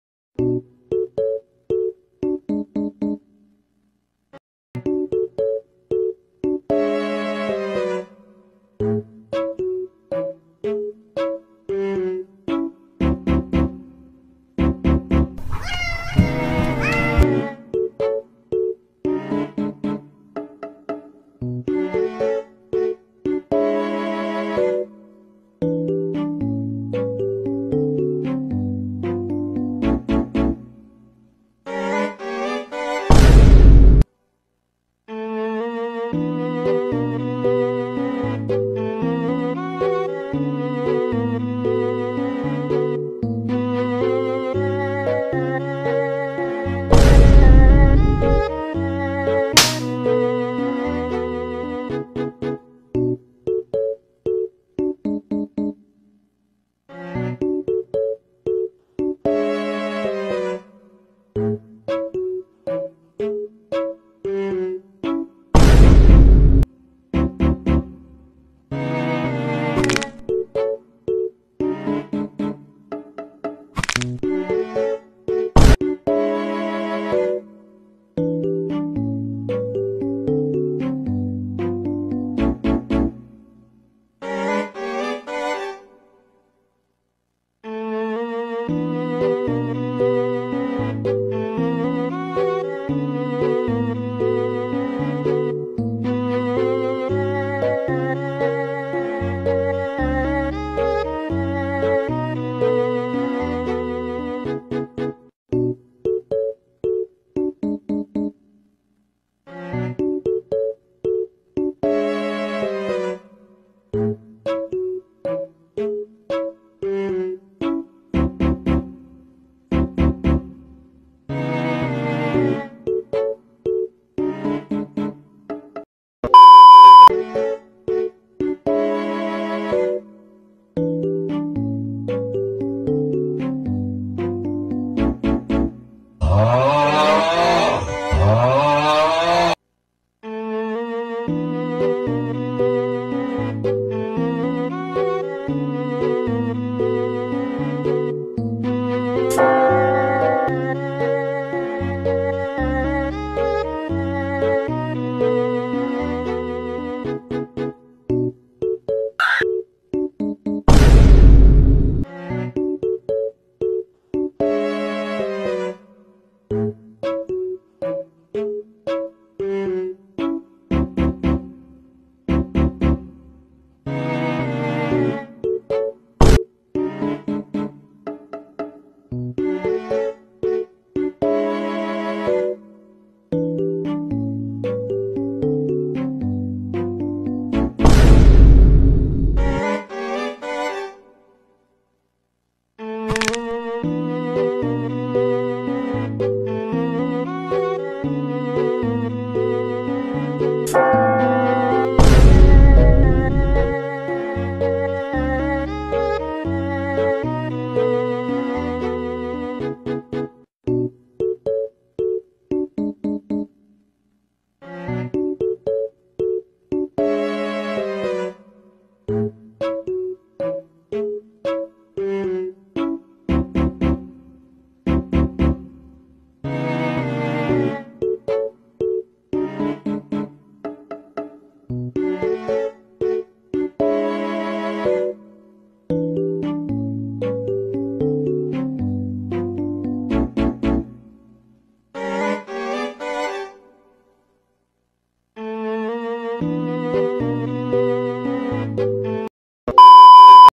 BSD does Your Best(worst) Dares! late 1k celebration//✨BSD( bungo stray dogs)✨//
TW:
gay homo's
bones cracking
Loud noises

Ships:
shin soukoku
soukoku
fyozai
fyolai?
Ranpo x yosano